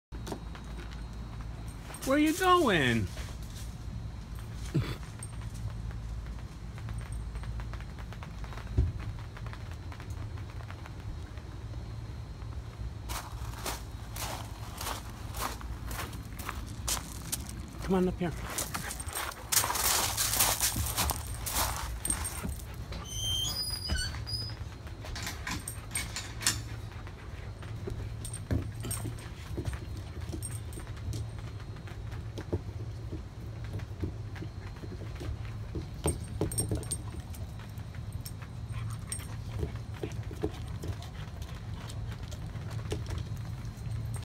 7/7 Murphy
Enjoying a cool off with some peanut butter ice cream from Taj AMA-Hound